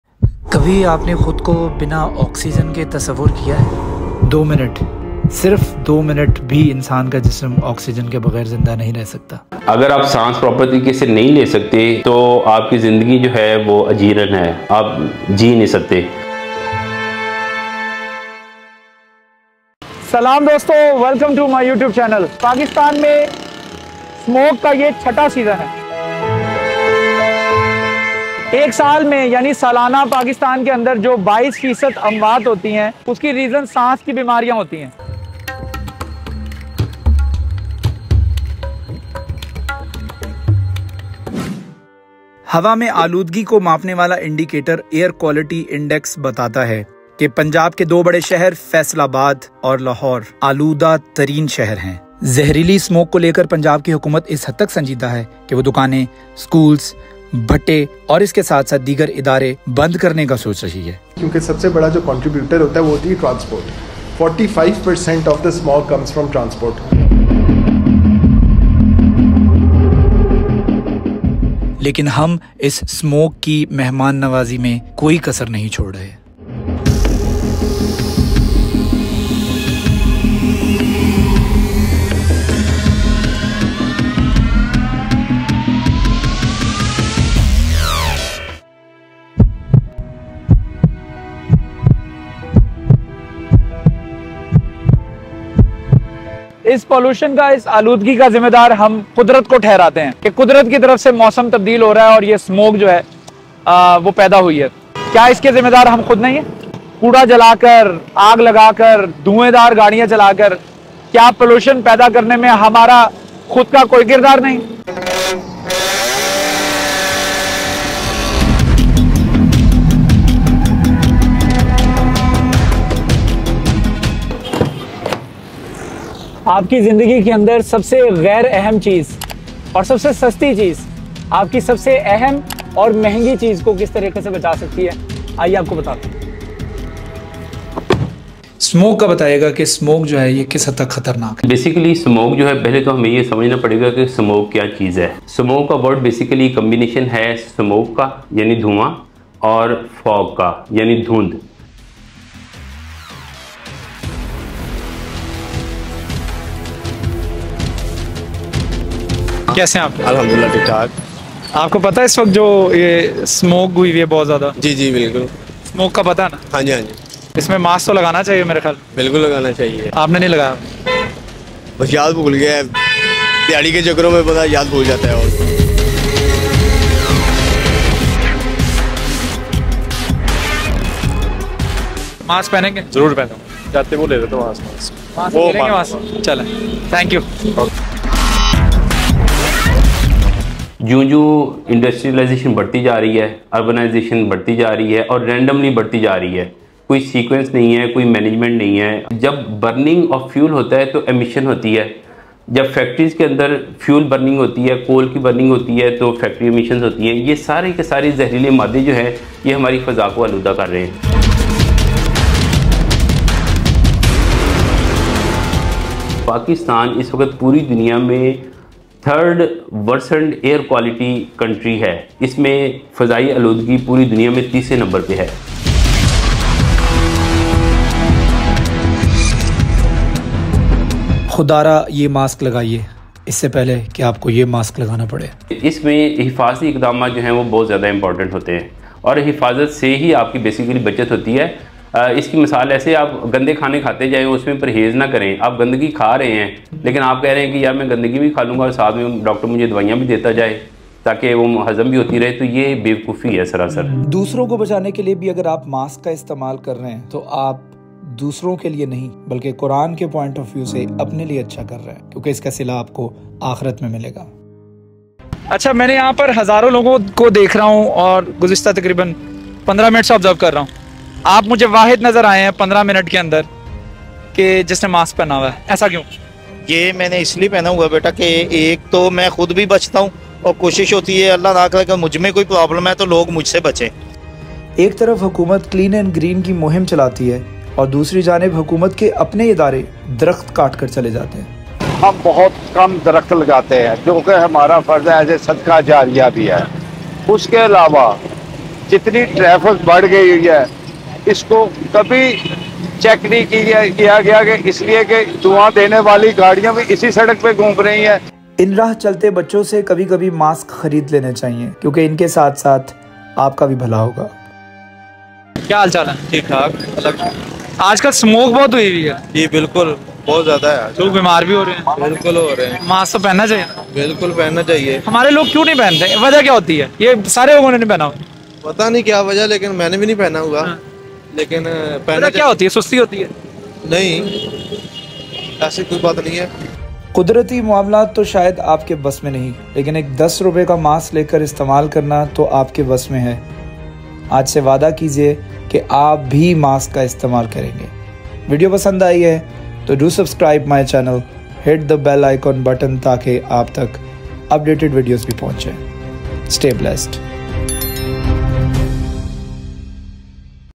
Caution ⚠️ A Slow Poison | Hidden Facts of Smog you Should Know
Facts about Smog are in length discussed in this video . Punjab which is a key Province of Pakistan is on high alert right now . Our government is struggling hard to minimize Smog and its impacts and trying to squeeze public transport as much as they possibly can but on the other hand public of Pakistan is not even aware of this Deadly smog and its impact . in this video I tried to aware and open the eyes of general public so that they can take care of themselves and their family from this life threatening smog .